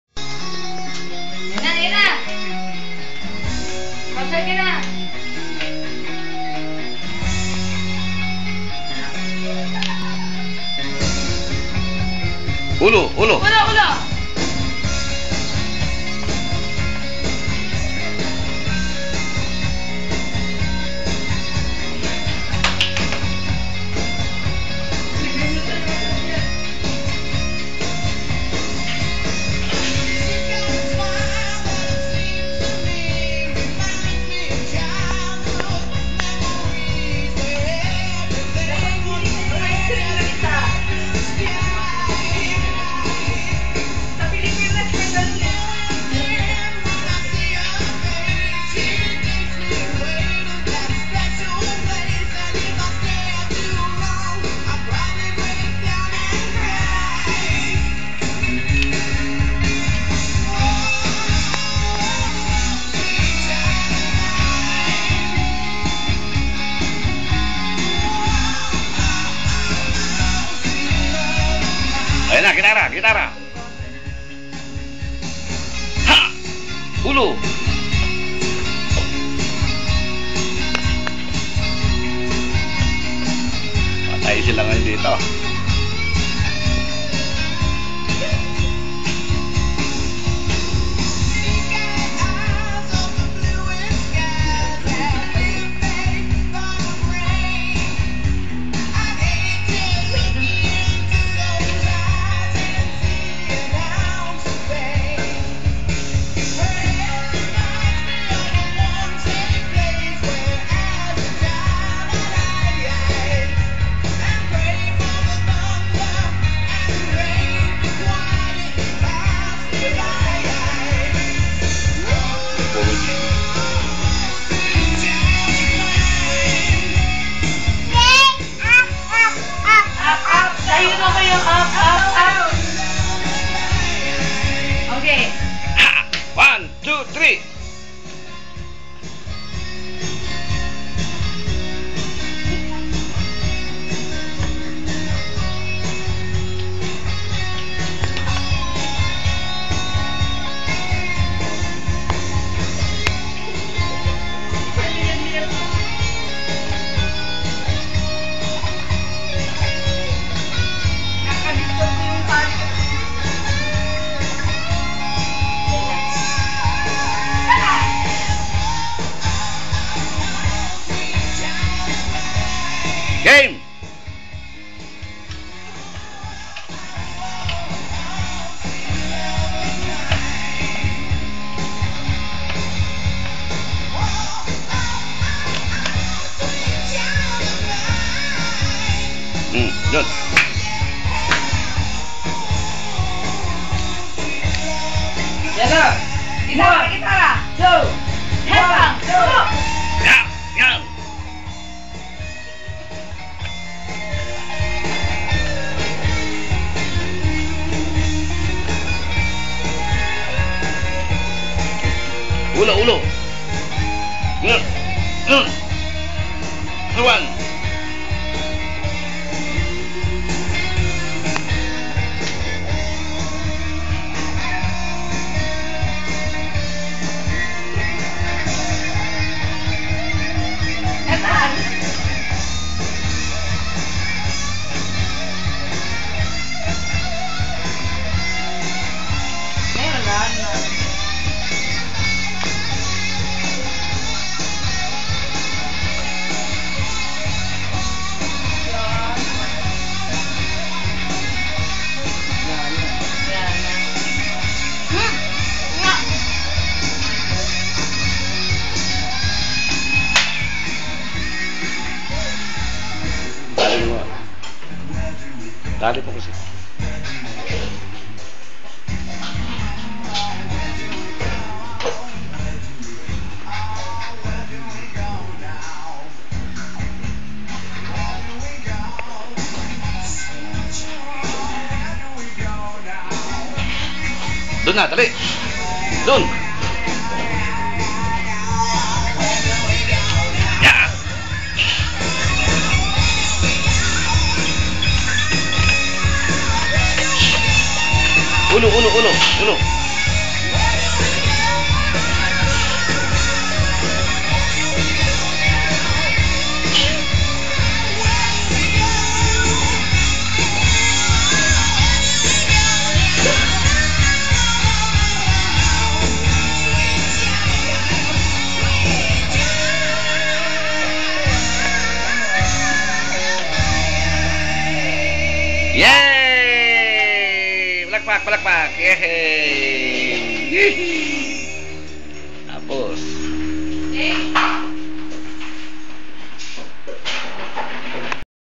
rainierbrice 1yr 8 months guitarist
my son playing guitar...